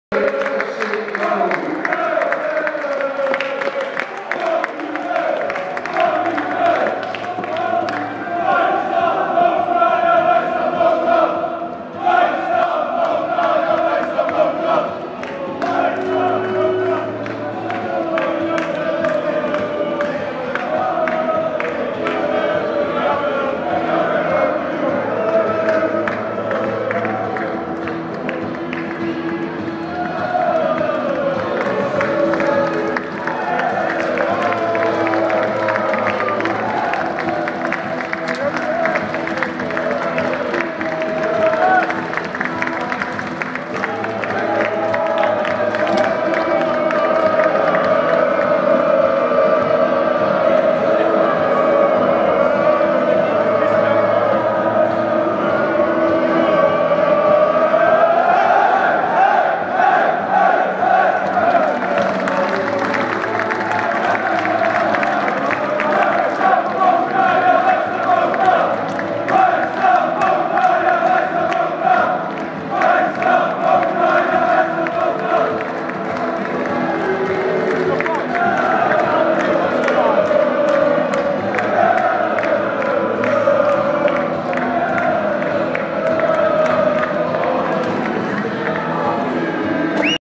FC Dordrecht bedankt publiek na 0-1 overwinning op Fortuna Sittard 22-11-2013
FC Dordrecht wint uit bij Fortuna Sittard met 0-1 door een goal van Rihairo Meulens in de 29e minuut.